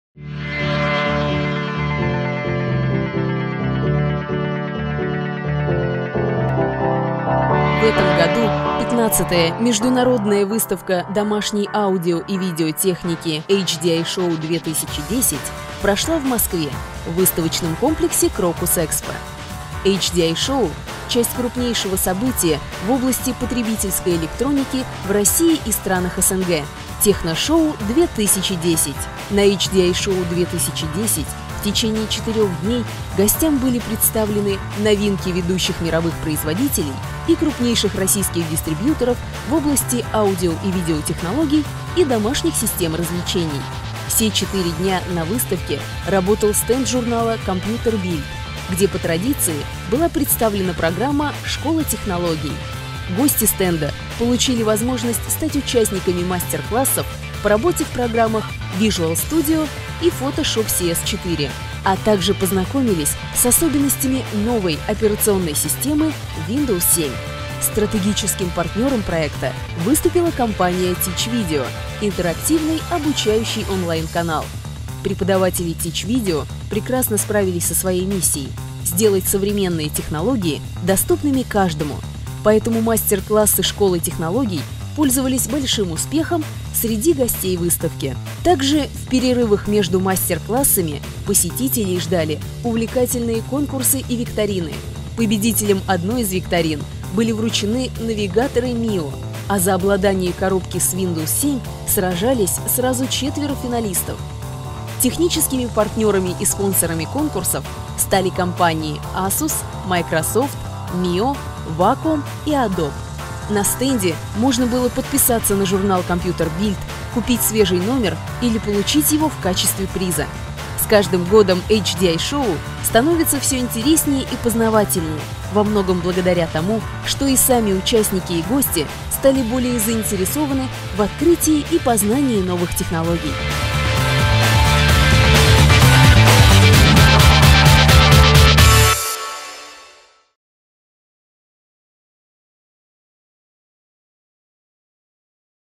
Репортаж с HDI Show-2010
С 15 по 18 апреля в Москве проходила известная международная выставка HDI Show-2010.

На HDI Show-2010 в течение 4 дней гостям были представлены новинки ведущих мировых производителей и крупнейших российских дистрибьюторов в области аудио- и видеотехнологий и домашних систем развлечений: 3D-технологии, домашние компьютеры, LED-телевизоры, видеопроекторы, Blu-ray и HD-проигрыватели, наушники, кабели, мебель для домашних кинотеатров и крепления для аппаратуры.

Наше участие в этой выставке -- проведение мастер-классов для всех желающих по популярным и новым программным продуктам в рамках проекта «Школа Технологий» совместно с журналом ComputerBild.
Так что смотрите наш репортаж с выставки и оставляйте свои мысли в комментариях!